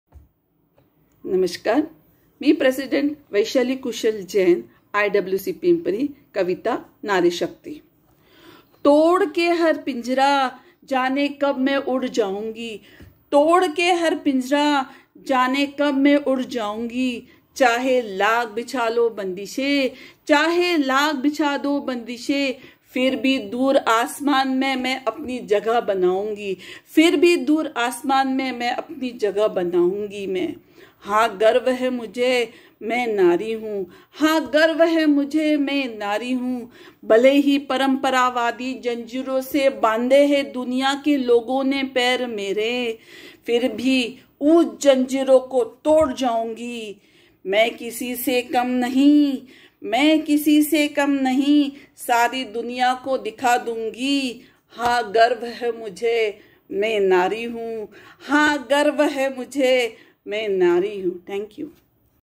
Pink First Short film contest. A short film on women empowerment
president Vaishali jain from Innerwheel club of pimpri dist 313
A poem on Narishakthi